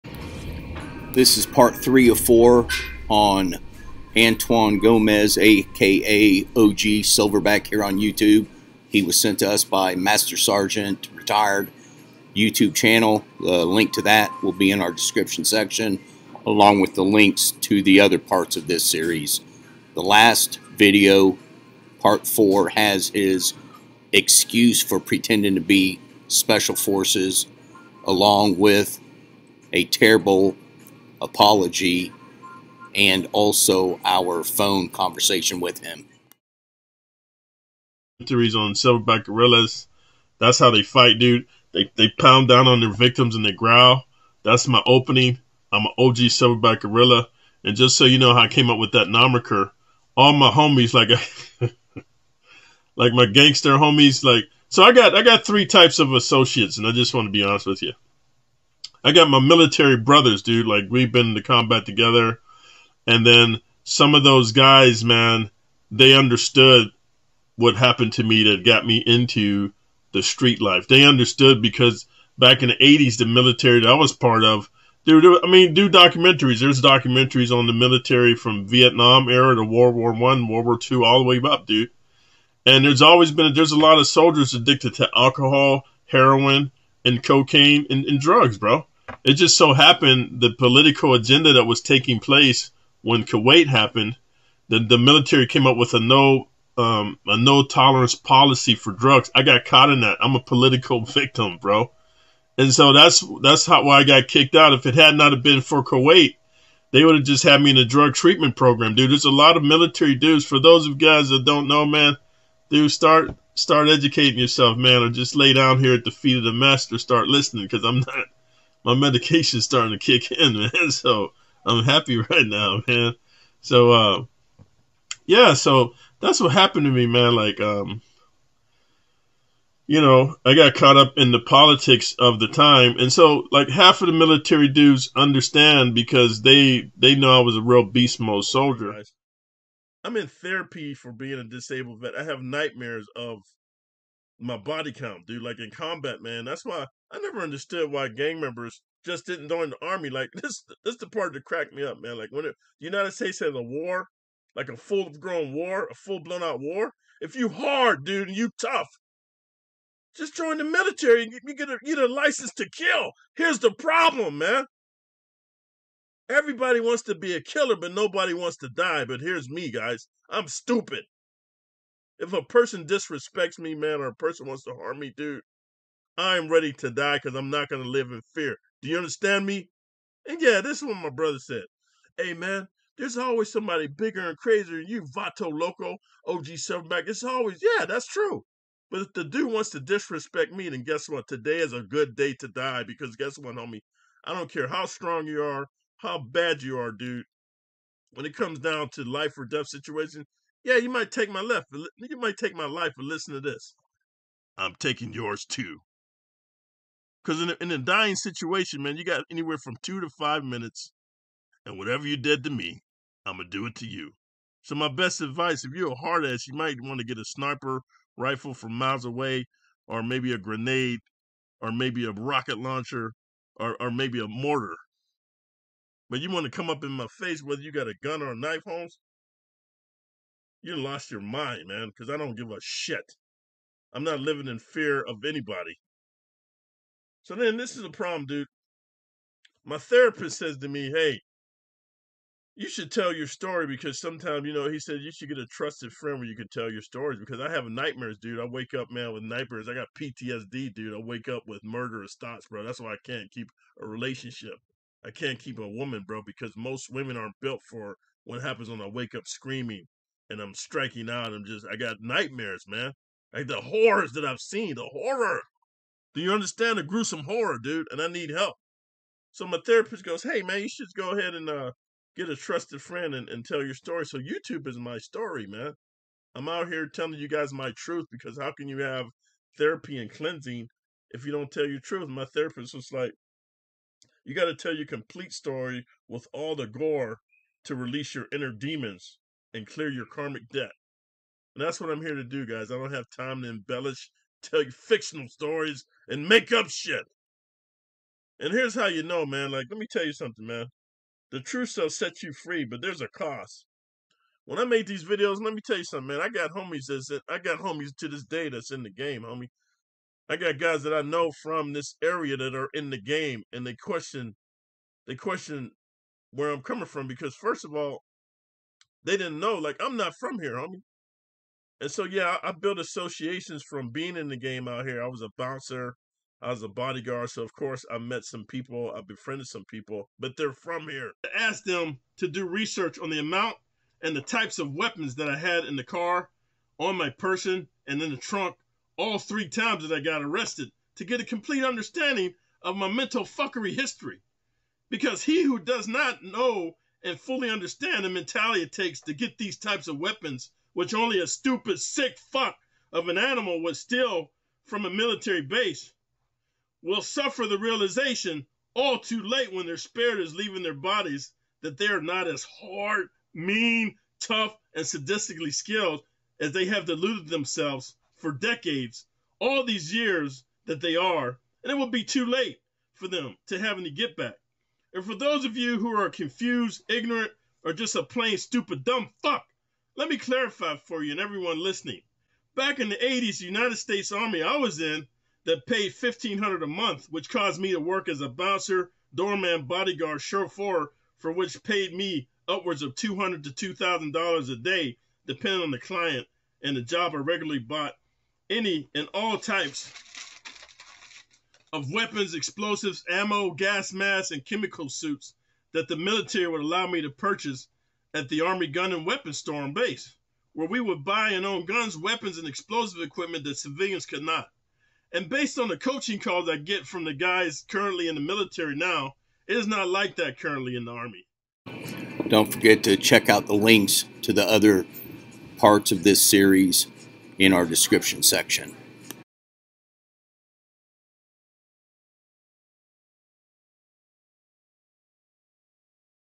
Antoine E. Gomez, Part III of IV, Airborne Ranger, Green Beret, Black Ops? Not so fast...
Antoine E. Gomez (OG Silverback on YT) was sent to us by the YT Channel MSG Retired. This is part III of a IV part series. We were sent many a video of him claiming Airborne, Ranger, SF, Black Ops.
When some Veterans called him out, he said he was close enough to graduating that he felt like it was ok to say he was one.
The only problem with that is he NEVER even attempted what he claimed he almost graduated.

Here's Part II of IV with more claims of him taking the SF route after he couldn't pass the AF ASVAB. LOL

Part IV of IV has his excuse that he had "almost graduated" so he thought it ok and also his phone call with us admitting he never even attempted any of what he claimed.